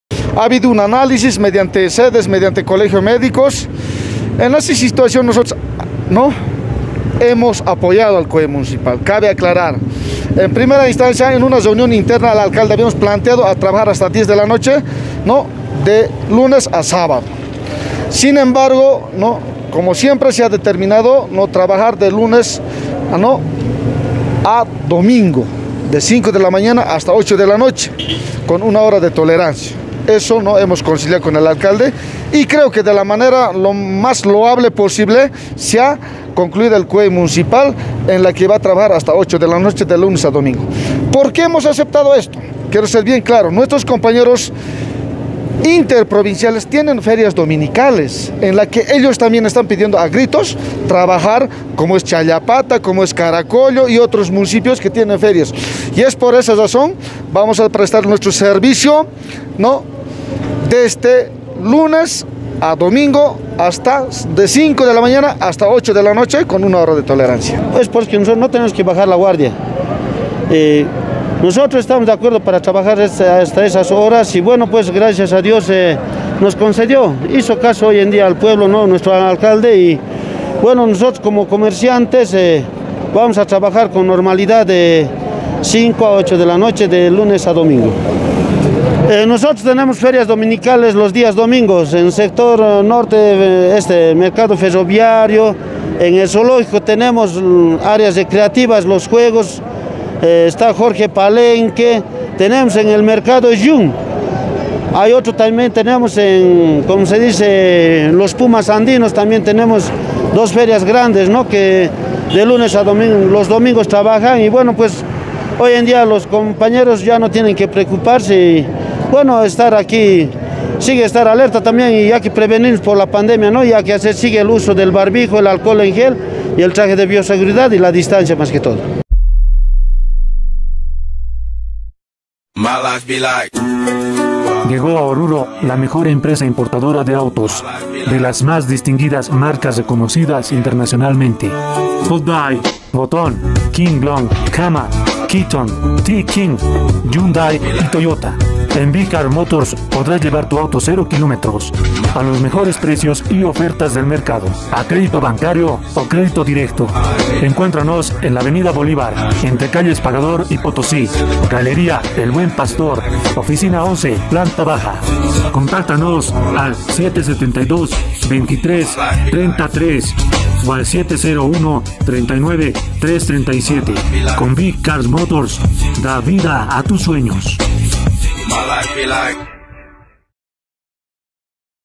COEM: TRANSPORTISTAS Y GREMIALES APOYAN AMPLIACIÓN DEL HORARIO LOS DOMINGOS
Tras la novena sesión del COEM , se determino ampliar el horario de circulación vehicular , peatonal además de las actividades económicas de lunes a domingo de 05:00 hrs. hasta las 20.00 hrs. , además de reanudar las actividades deportivas, religiosas y culturales con todas las medidas de bioseguridad.